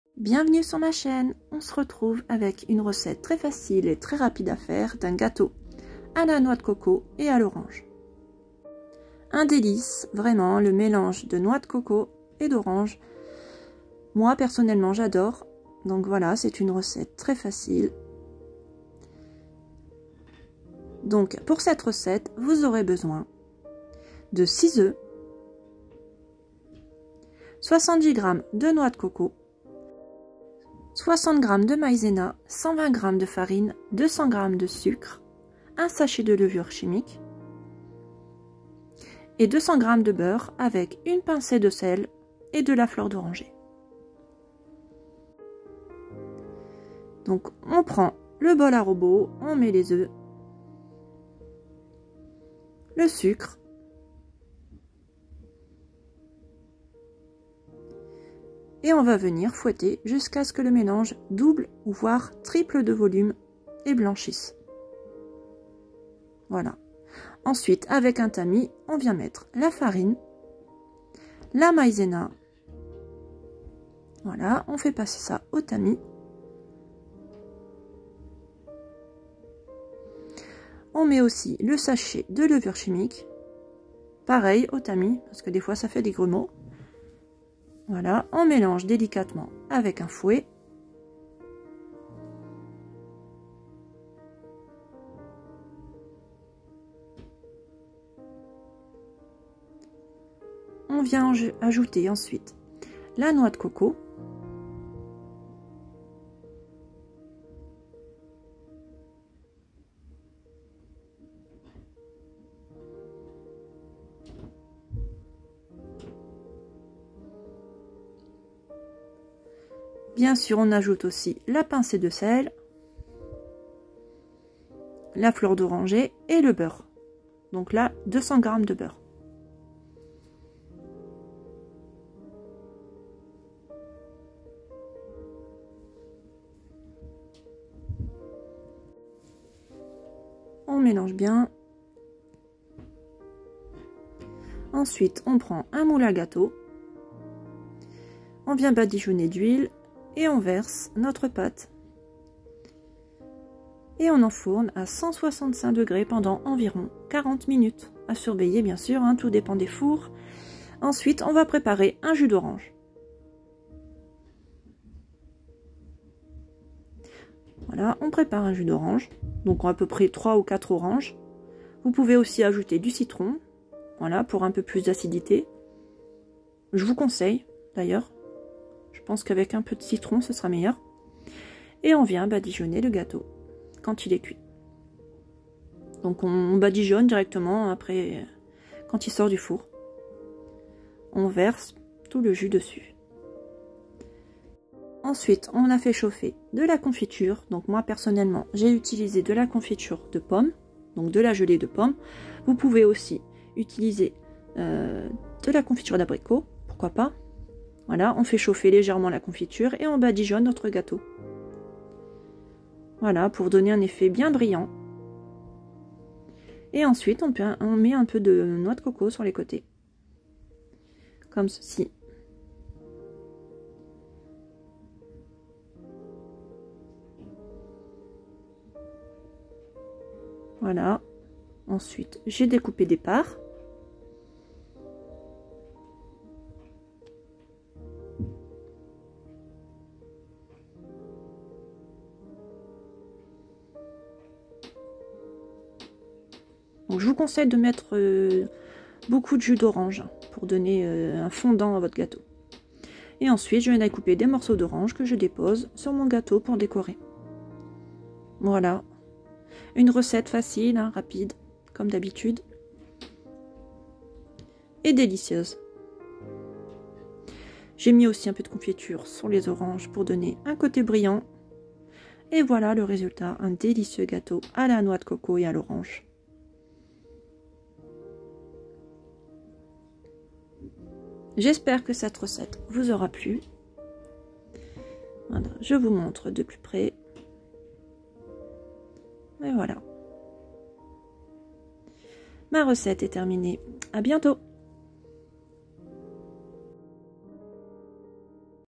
🍊🥥Gâteau à la Noix de Coco et à l'Orange Recette Facile et Rapide
👩‍👦Je suis une maman qui aime cuisiner et partager mes recettes et je vous propose une recette de gâteau à la noix de coco et à l'orange.   🥥🍊Les Ingrédients Pour La Recette Sont : 6 œufs ,70 gr de noix de coco ,60 gr de maïzena ,200gr de beurre ,200 gr de sucre ,de la fleur d'oranger, 120gr de farine ,sel ,1 sachet de levure chimique, du jus d'orange et de la confiture. 🌡 cuire à 165 degrés pendant environ 40 minutes.